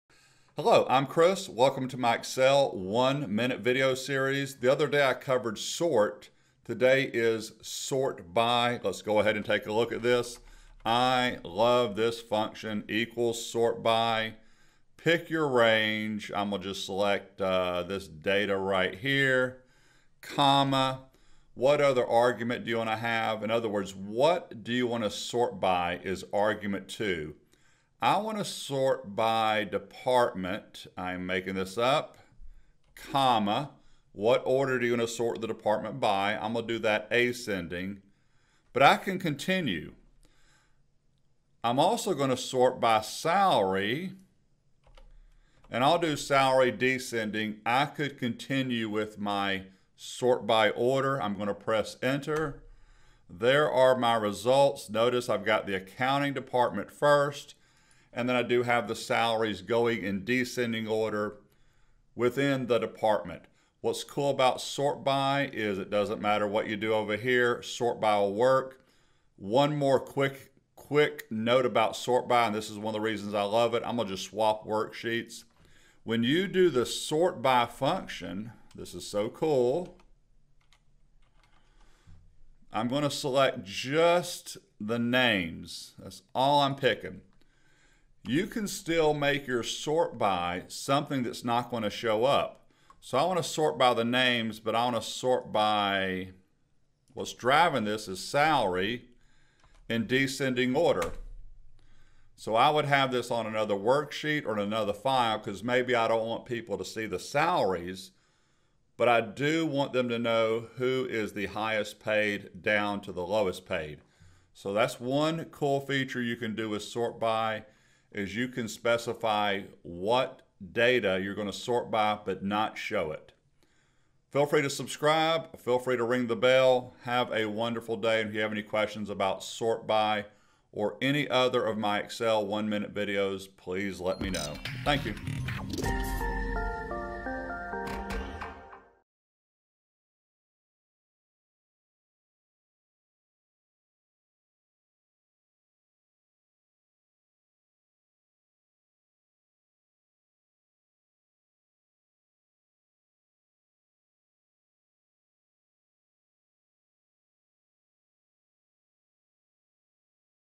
Excel SORTBY Function | Excel One Minute Functions Explained
The SORTBY function in Excel sorts the contents of a range or array based on the values in a corresponding range or array. Use SORTBY to see a list of employees sorted by highest paid, but you don't show their salaries. USE SORTBY to sort a customer list by the most profitable customer but not see the revenue. SORTBY has two required arguments, but you can sort by multiple criteria. You can use the SORTBY function on another worksheet or on another file or workbook.

Syntax
=SORTBY(array, by_array1, [sort_order1], [by_array2, sort_order2],…)

Example
=SORTBY(B2:C27,F2:F27,-1)

Argument Description
array - The array or range to sort - Required
by_array1 - The array or range to sort on - Required
[sort_order1] - The order to use for sorting. 1 for ascending, -1 for descending. Default is ascending.
[by_array2] - The array or range to sort on
[sort_order2] - The order to use for sorting. 1 for ascending, -1 for descending. Default is ascending

Other videos on the SORTBY function by Chris Menard

Versions of Excel the SORTBY function works with
Excel for Microsoft365
Excel for Microsoft 365 for Mac
Excel for the web
Excel 2021
Excel 2021 for Mac
Excel for iPad
Excel for iPhone
Excel for Android tablets
Excel for Android phones

-- EQUIPMENT USED
○ My camera